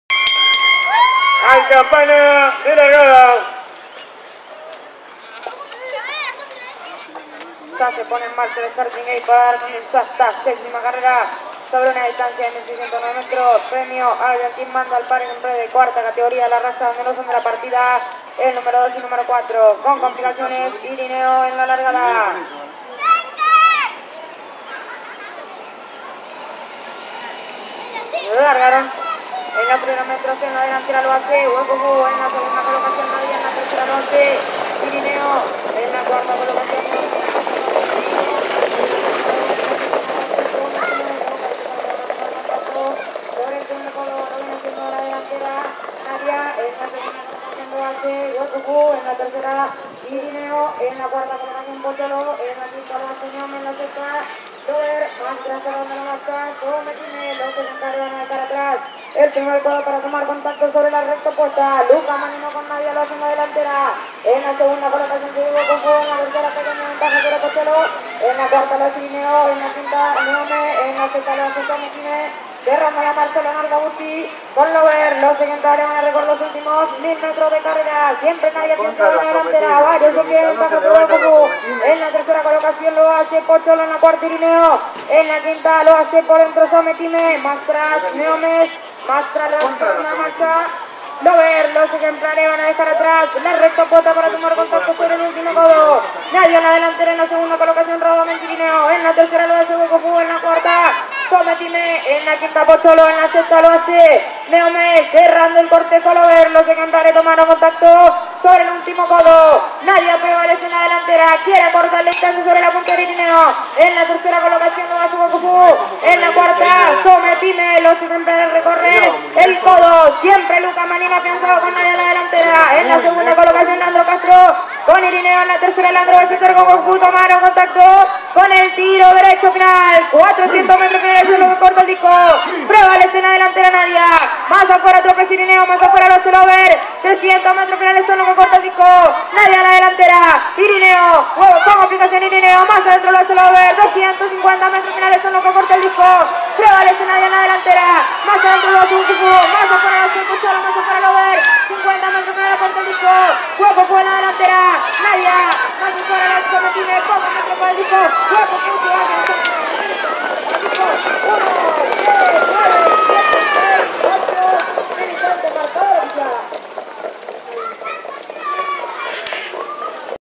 Hipodromo De Hurlingham Reunion N°20 carrera N°7 15-06-2014
Tr.Fresh Huecunfu - Driver: Leandro Ballester - Stud Don Miguel
Distancia 1609 mts. - Tiempo: 2'13' '40 - Promedio: 1'22''9.-